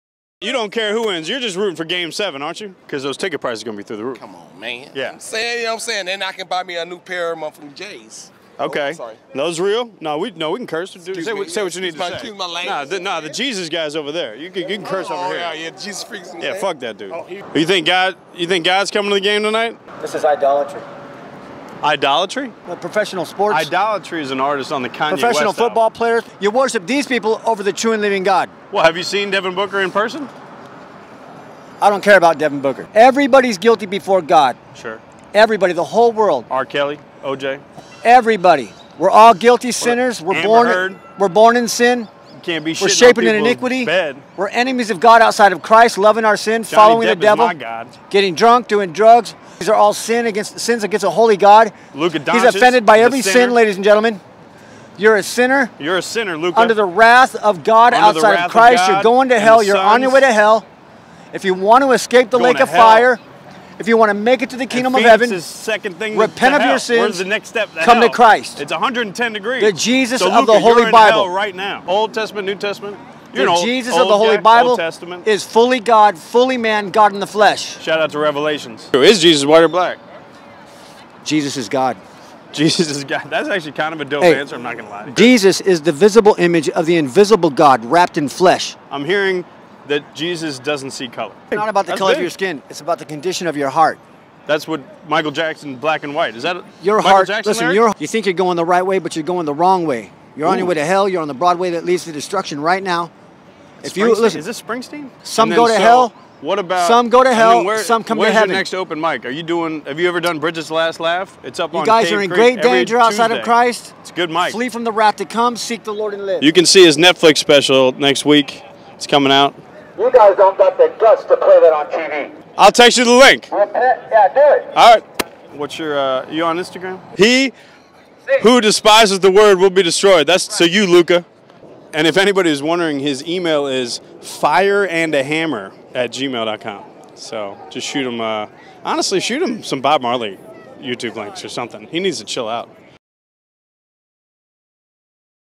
Harassing the Jesus Guy at Suns Game
Comedian Michael Turner harasses the Jesus guy that harasses people before the Phoenix Suns NBA Playoff game vs the Dallas Mavericks